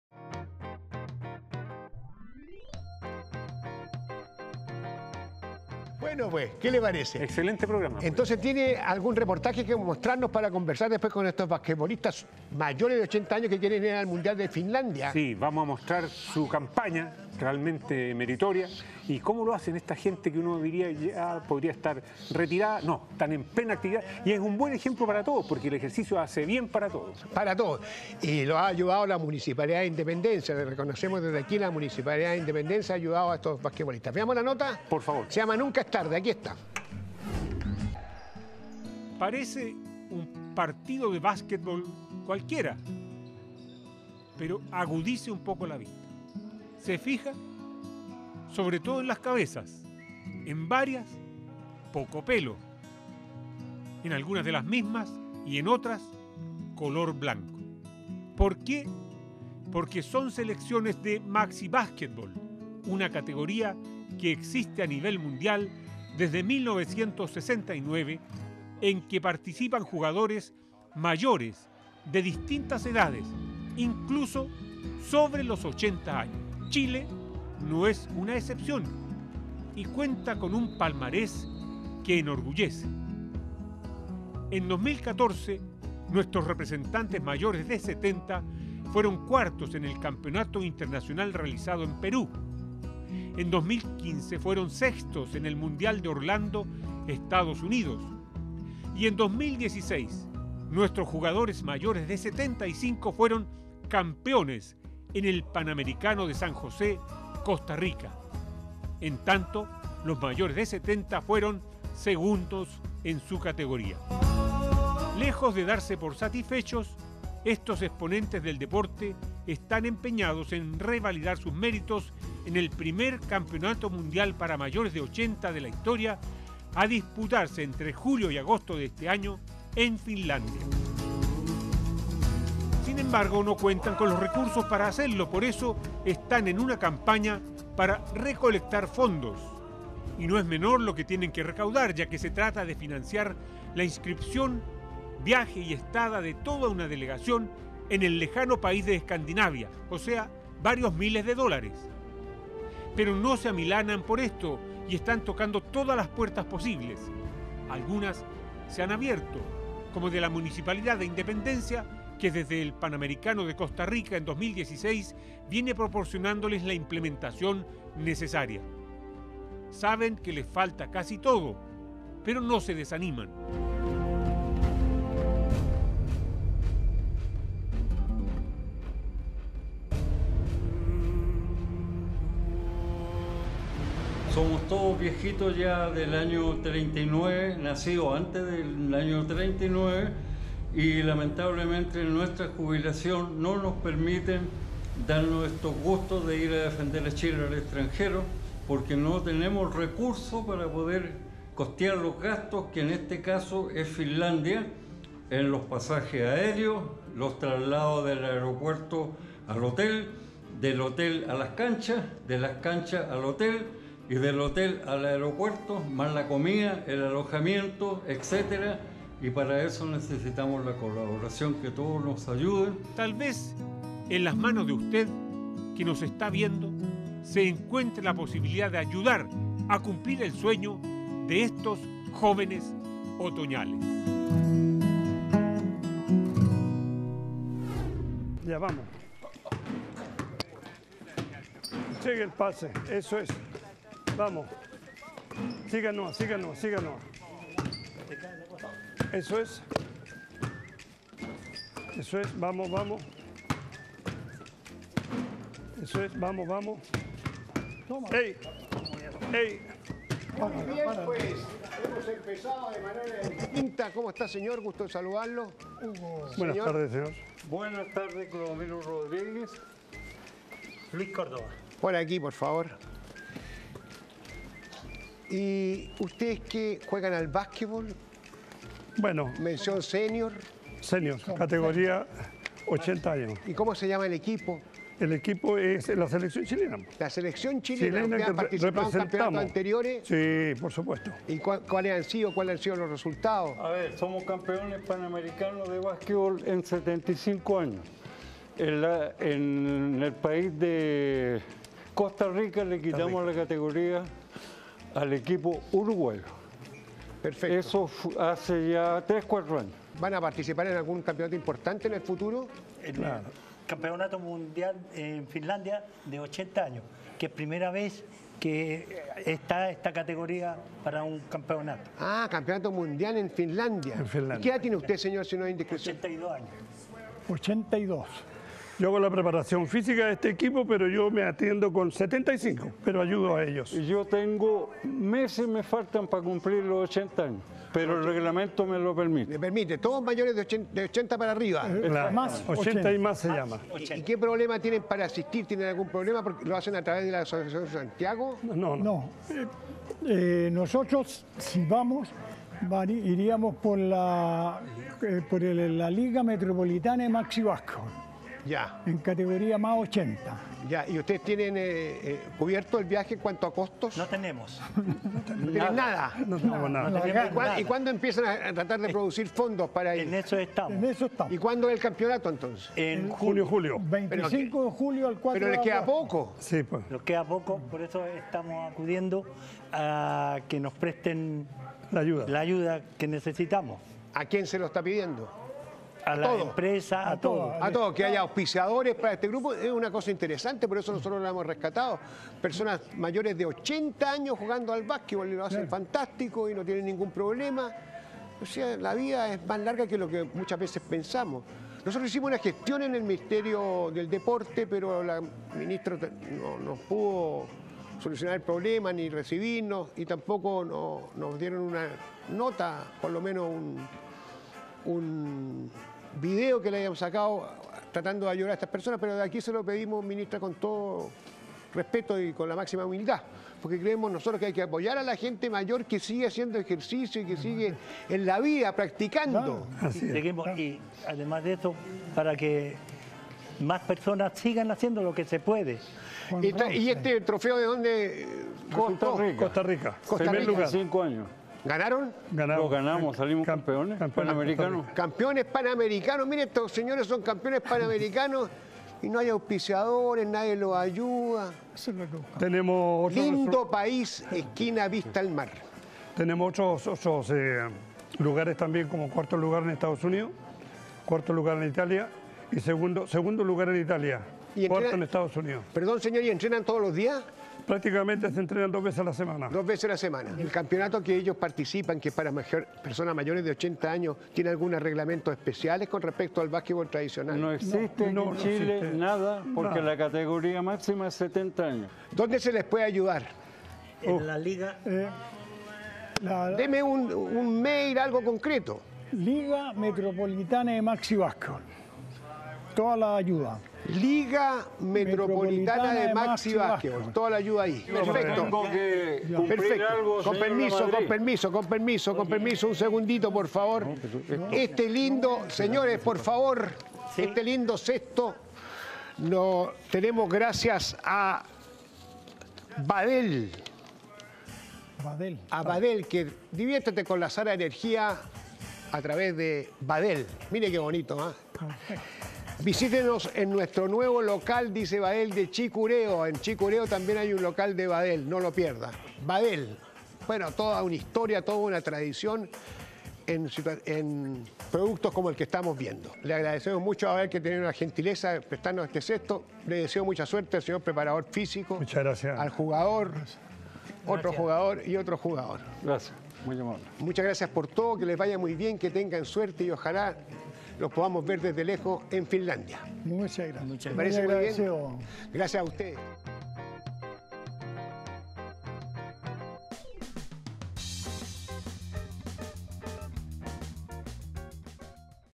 Maxi basquetbol sobre 80 años CAMPEONES CADA DÍA MEJOR TV
¡Nunca es tarde! Ayudemos a estos basquetbolistas mayores de 80 años, necesitan financiamiento para representar a nuestro país en el próximo campeonato mundial de Finlandia. Excelente reportaje de Gonzalo Rojas.